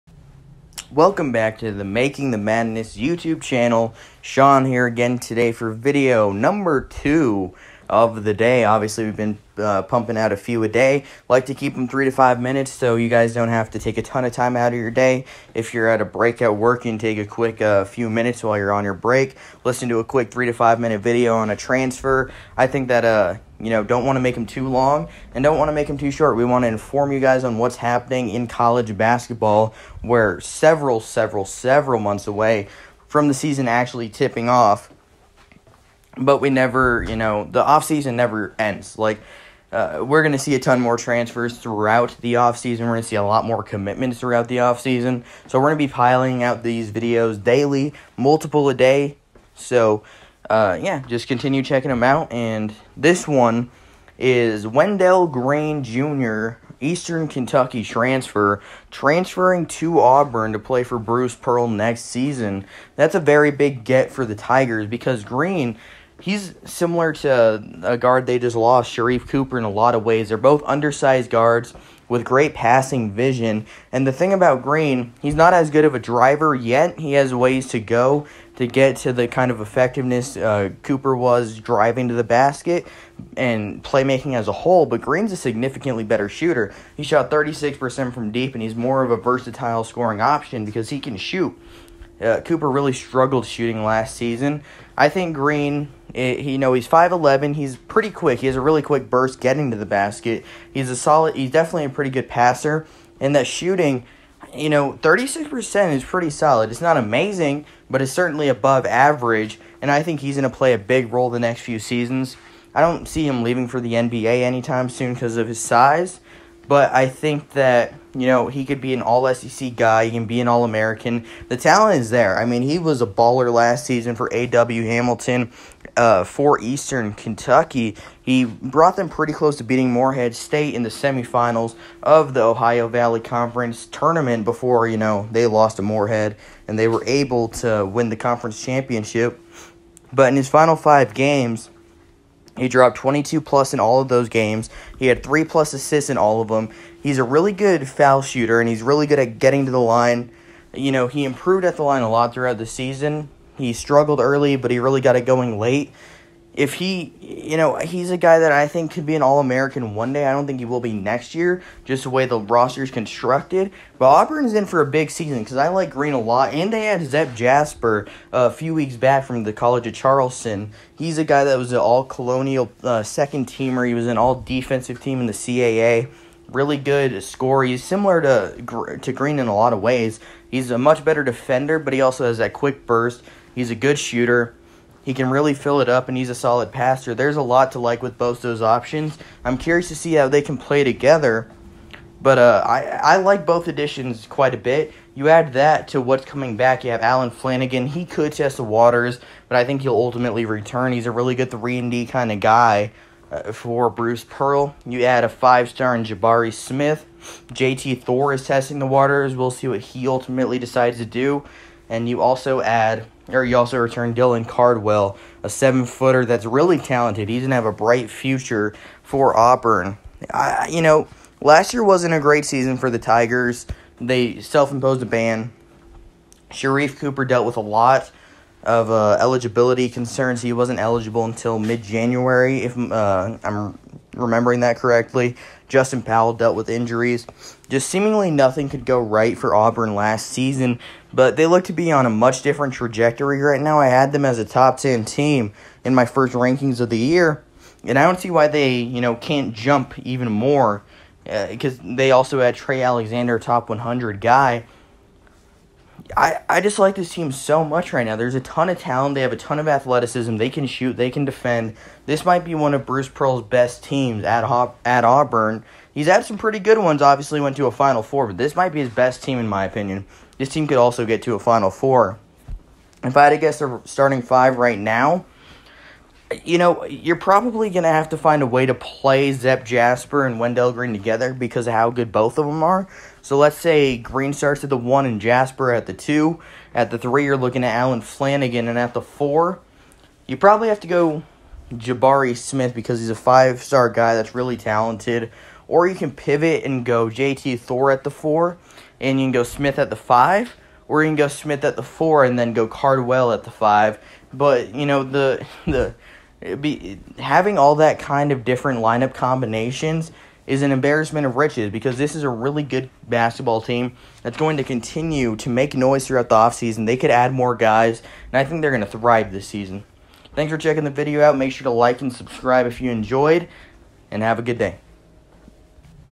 Wendell Green is Auburn bound
Sean breaks down the news of Wendell Green deciding to head to Eastern Kentucky and what it means for the Tigers next year.